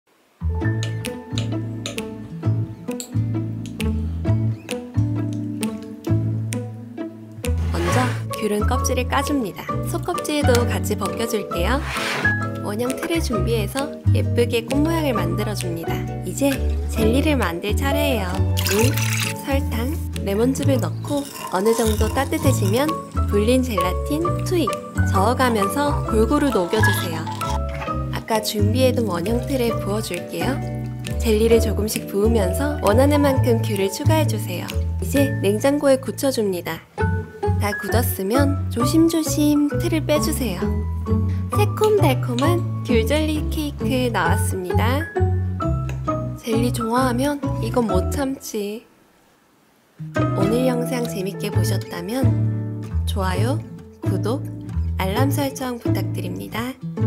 탱글탱글 귤젤리 케이크 만들기 | Tangerine Jelly Cake Recipe | 대왕쁘띠첼 레시피 | 과일푸딩 만드는법
새콤달콤한 귤젤리 케이크 레시피입니다 :)
저는 귤 껍질을 하나하나 까서 사용했는데,
귀찮으시다면 시판 귤 통조림을 쓰셔도 좋아요

Tip!
판젤라틴은 찬물에 10분정도 불린 후 꼭 짜서 사용하시면 됩니다
만약 조금 더 탄력있는 젤리를 선호하시는 분들은
불린 젤라틴을 2~3장 추가해주세요!

This is a sweet and sour tangerine jelly cake recipe :)
I used tangerine peels one by one,
If it bothers you, you can use commercially available canned tangerines.

Tip!
Soak gelatin in cold water for about 10 minutes and then squeeze it out before use.
If you prefer a more elastic jelly,
Add 2-3 pieces of soaked gelatin!